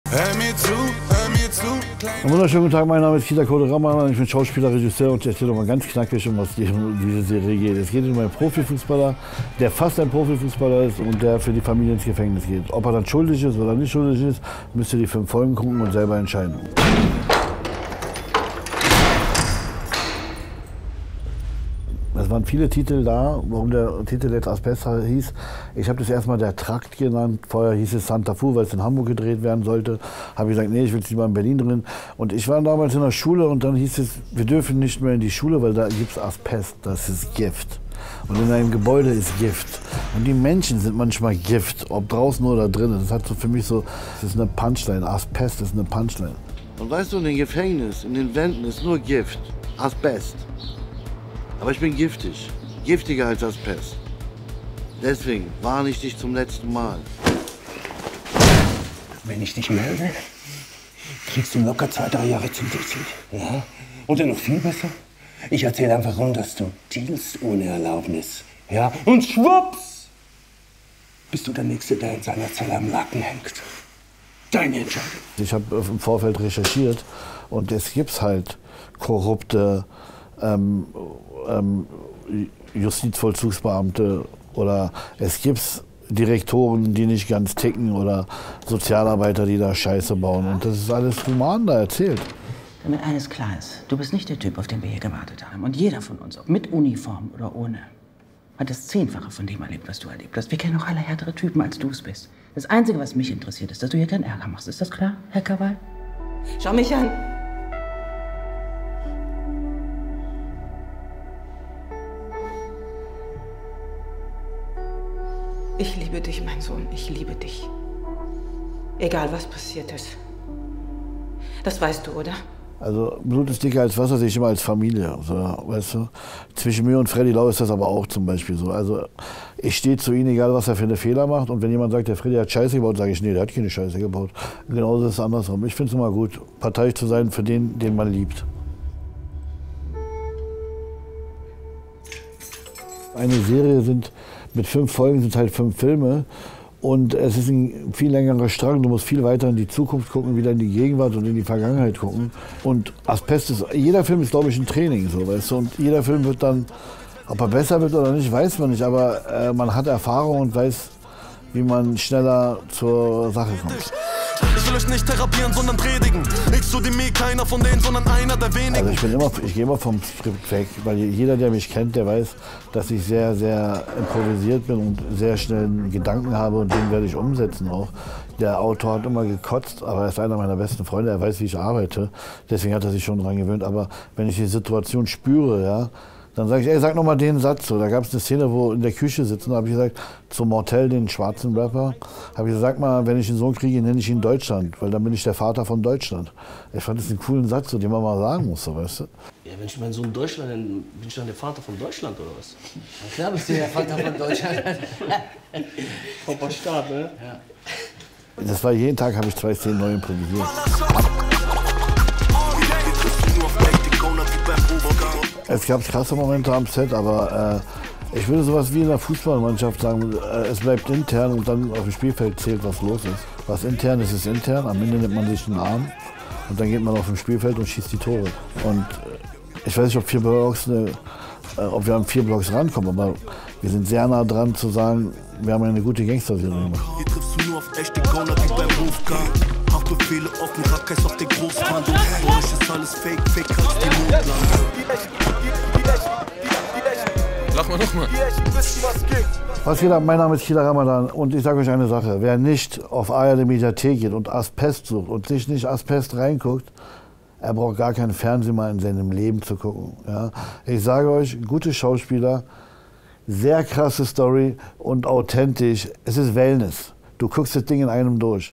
Vater von Deutschland? Deshalb würde Kida Ramadan seinem Kind diesen Namen geben
Kida Ramadans neue Serie "Asbest" ist ein voller Erfolg. Er hat uns erzählt, was Familie für ihn bedeutet, wie die Serie ihren Namen bekommen hat und warum Improvisation geil ist.